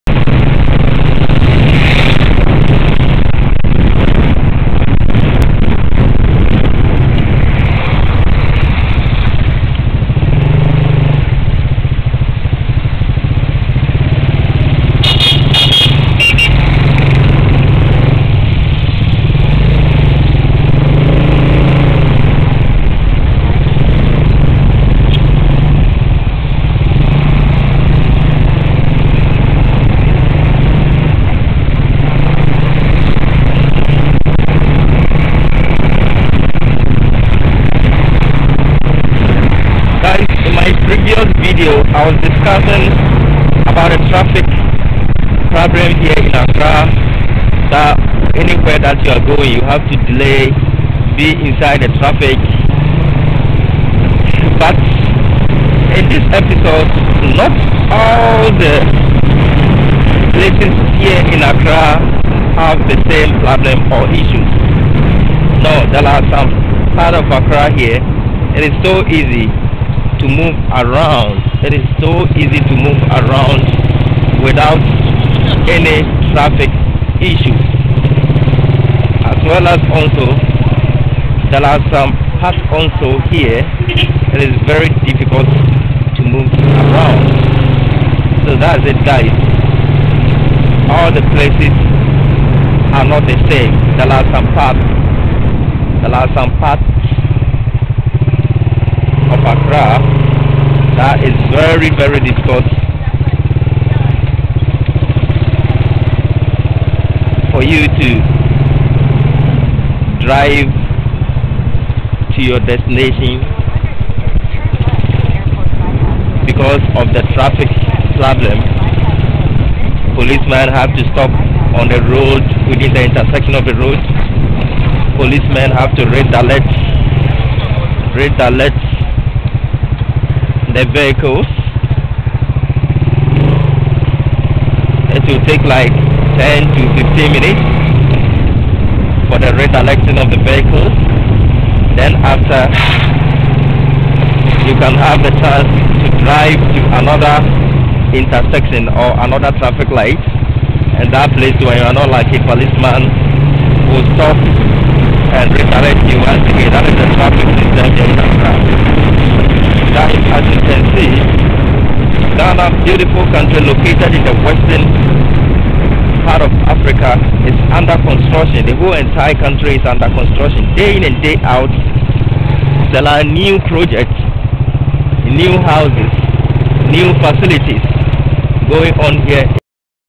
Episode 21 Accra City Life // Road Network in Airport-city Accra Ghana.
Airport City Accra is the most convenient location to settle and do business. Grow and Expand at the Airport City. We promote Airport City and attract businesses, events and visitors to the city.
in this video let as take a look at  the road network system in airport city.